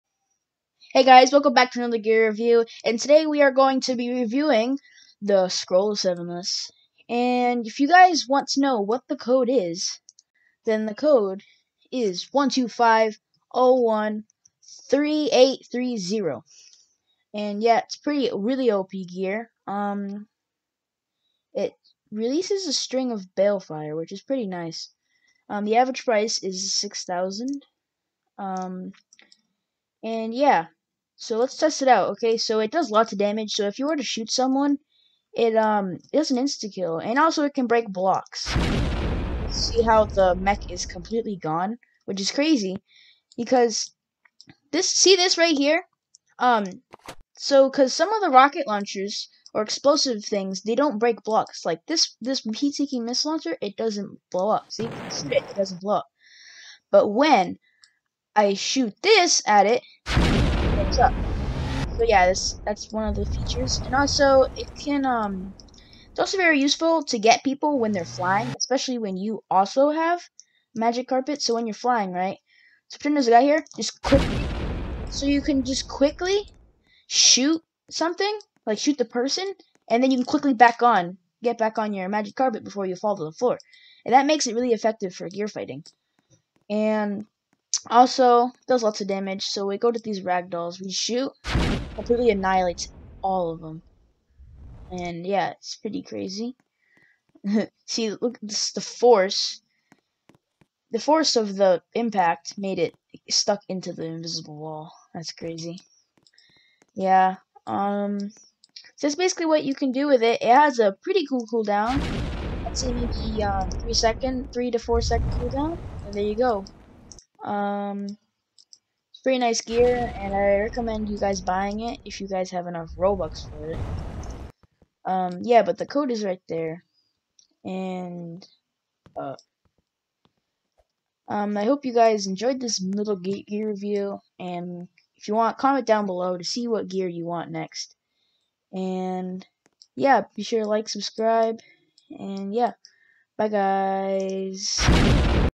Gear Review: Scroll of Sevenless
Hey guys, today I'm going to be reviewing the Scroll of Sevenless it is overall a very popular gear for its special balefire attack. It's worth over 6,000 robux.